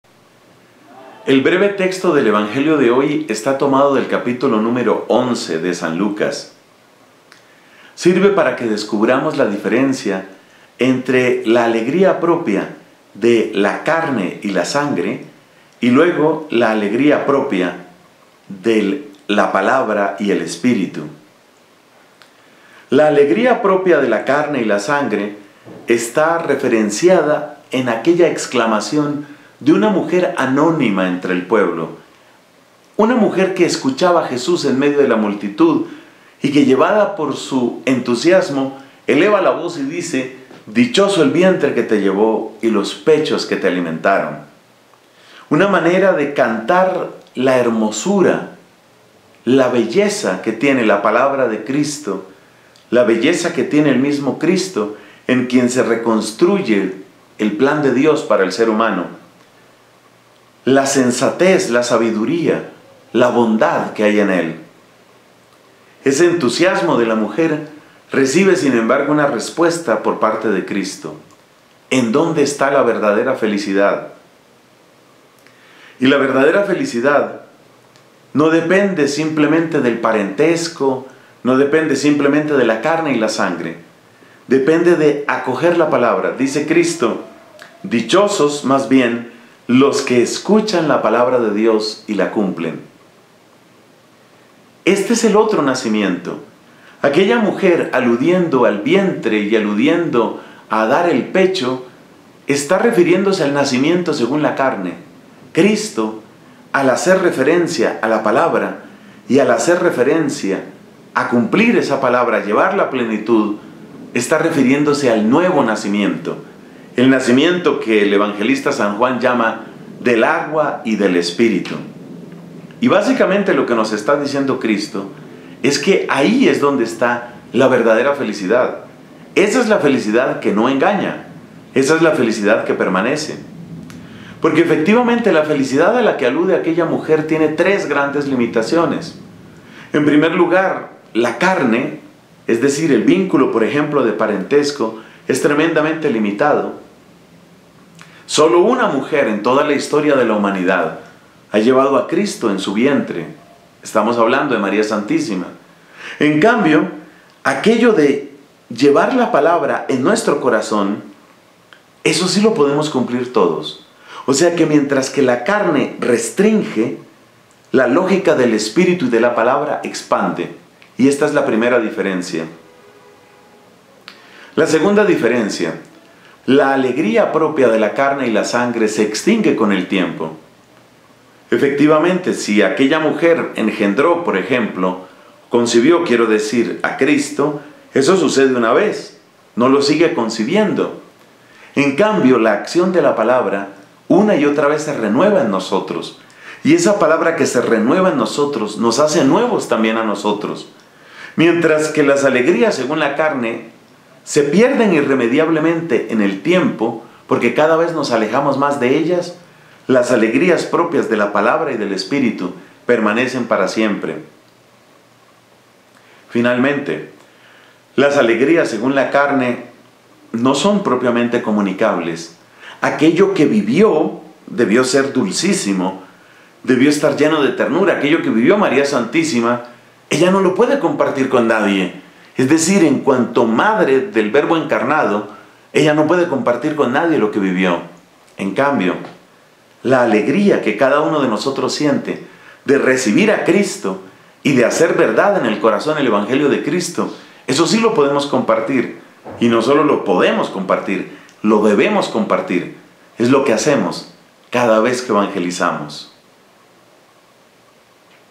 LA GRACIA del Sábado 10 de Octubre de 2015
La verdadera felicidad depende de acoger la Palabra, llevarla en nuestro corazón haciéndonos nuevos, y compartir con los demás a través de la evangelización.